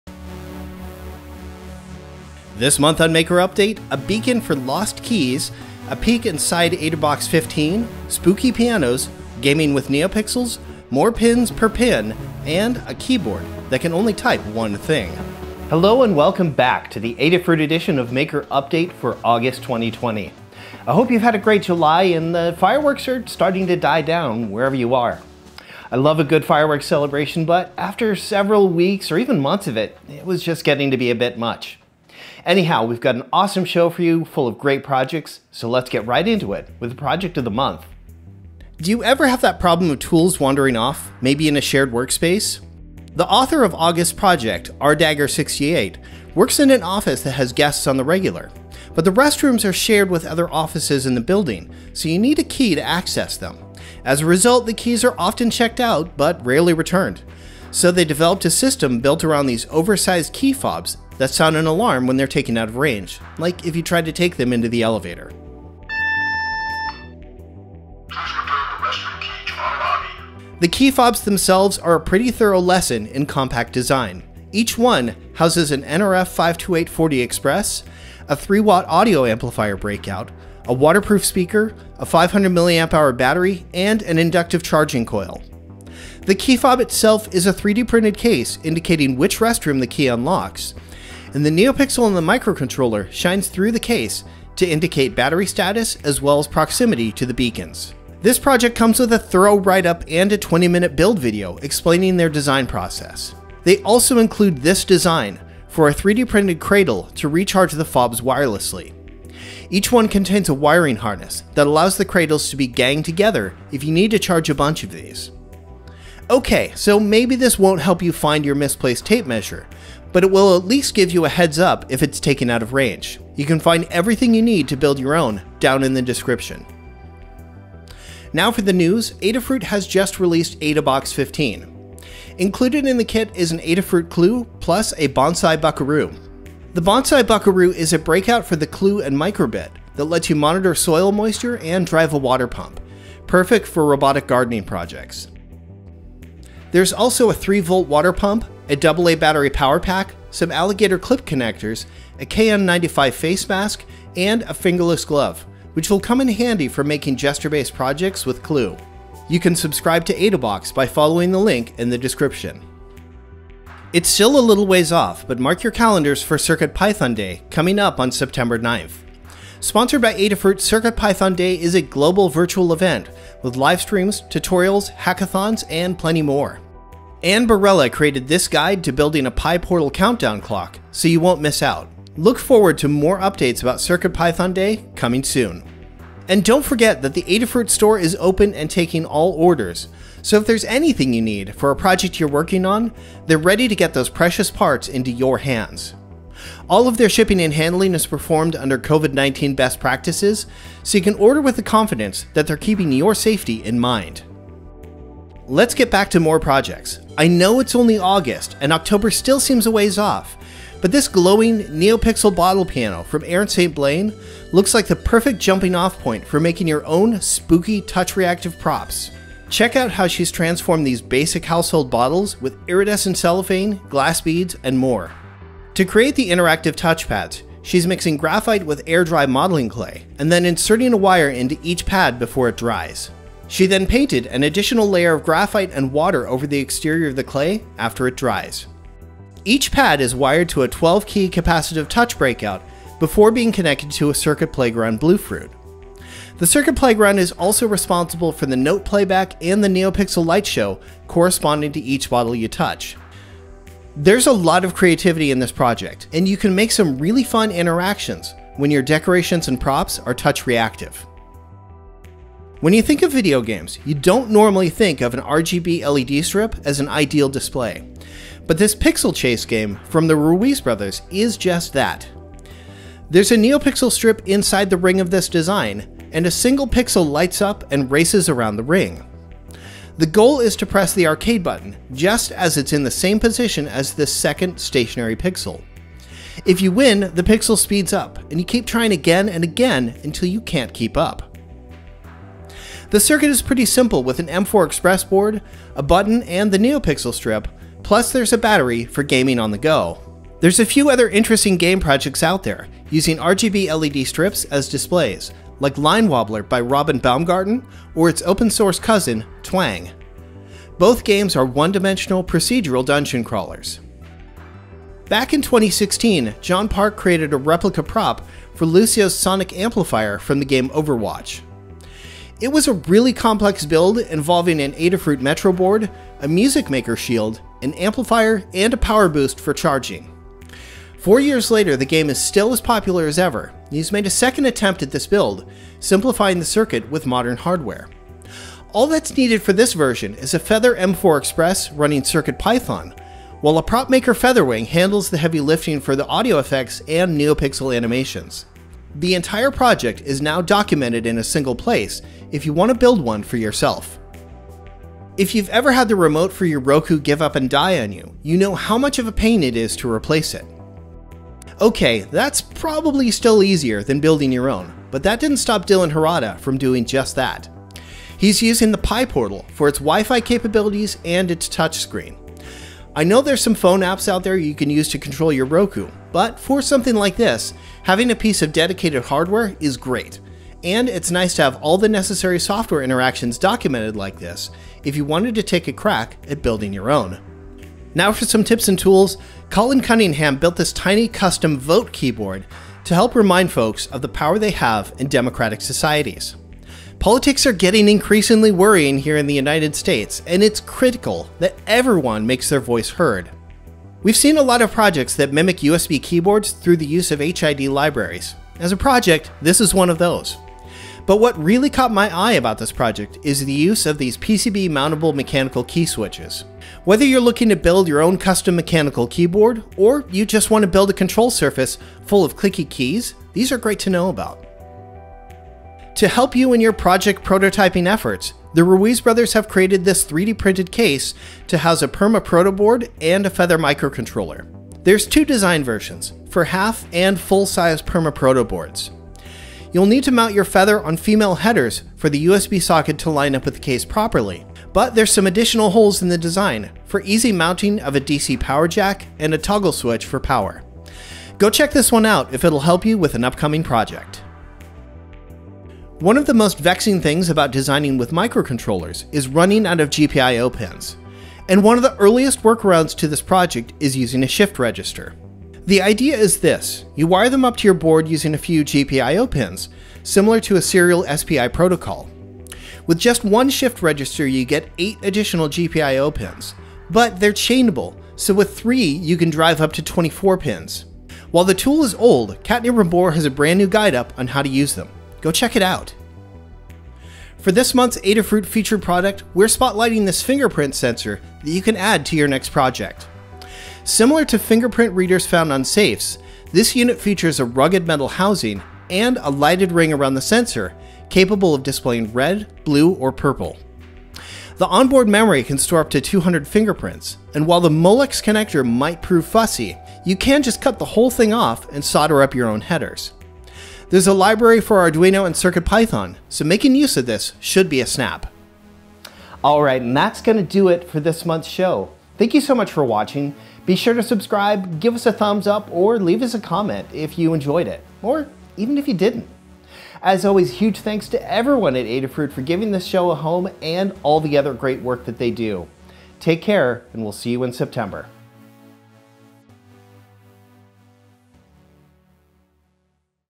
Maker Update #190: Keyed In Adafruit
Keyed In [Maker Update #190] *Adafruit Edition*

This month on Maker Update: a beacon for lost keys, a peek inside Adabox 15, spooky pianos, gaming with neopixels, more pins per pin, and a single purpose keyboard.

Show Notes

-=Project of the Month=-

-=News=-

-=Adafruit Projects=-

-=Contributed Projects=-

-=Tools/Tips=-

-=Product Spotlight=-